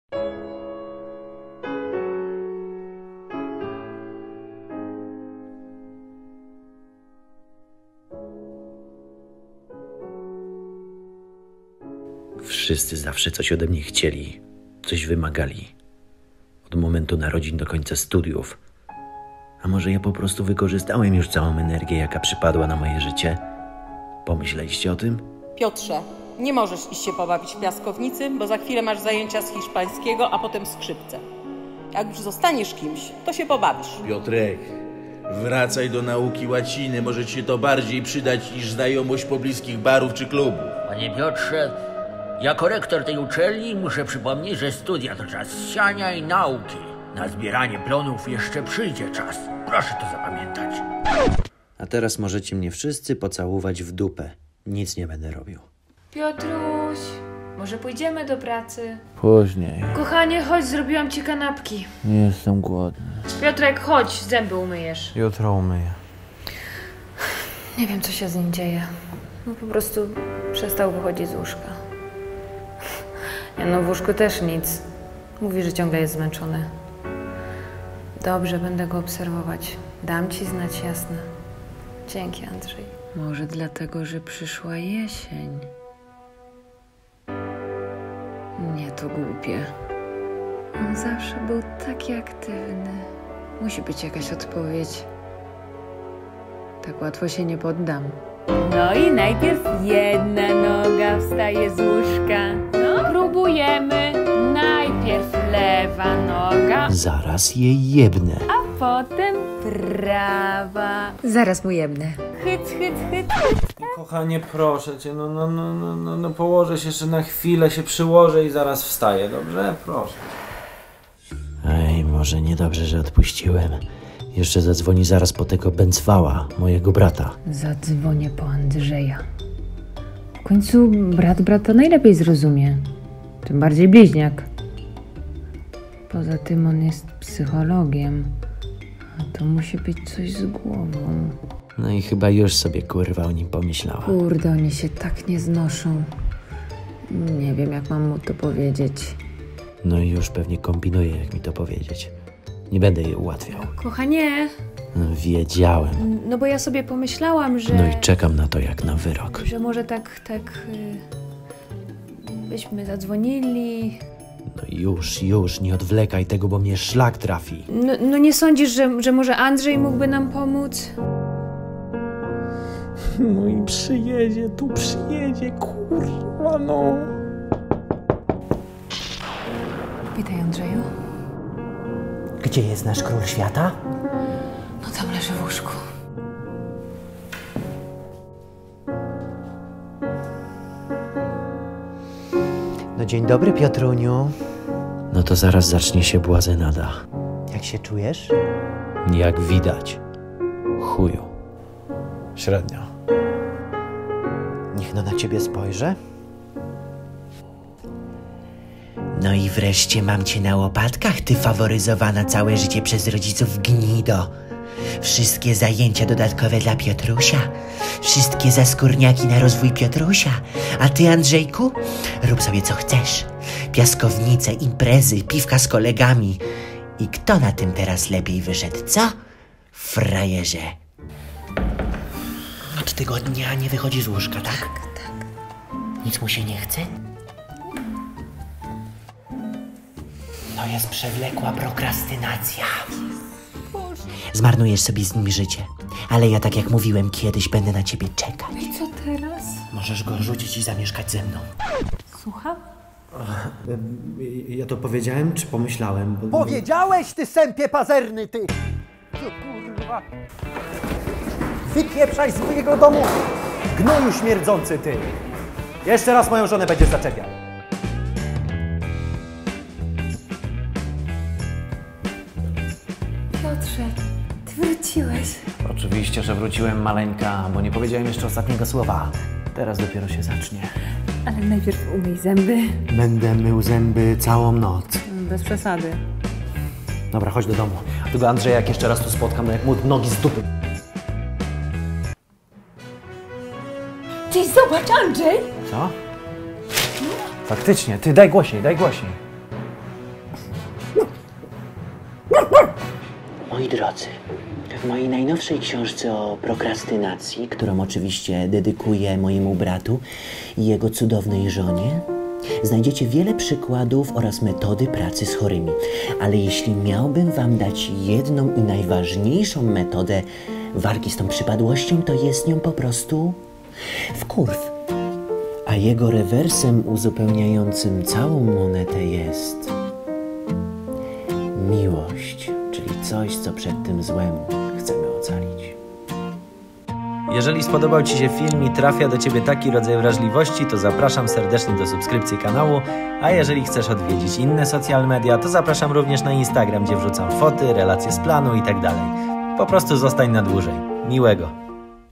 ŻYCIOWE WYPALENIE 😩
Czy też odczuwasz życiowe wypalenie? Są na to pewne niezawodne sposoby. Nie daj się jesiennej depresji. Obejrzyj film i zacznij działać!

Reżyseria: Sebastian Jasnoch
Zdjęcia: Jasiek Kędzia
Scemariusz: Mariusz Babicki 😂
Montaż: Sebastian Jasnoch
Kolor, stabilizacja: Jasiek Kędzia
Pomoc na planie: Wojciech Kaliński
Wystąpili: Paulina Nadel, Sebastian Jasnoch x2

Głosy z offu:
Mama - Ewa Nadel
Ojciec - Michał Floriańczyk
Rektor - ...Sebastian Jasnoch (ten sam co grał brata, ale inny który montował...ale się znają)